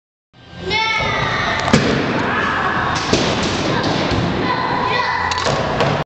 Tera Bowling
Tera giving her best shot...or is it her worst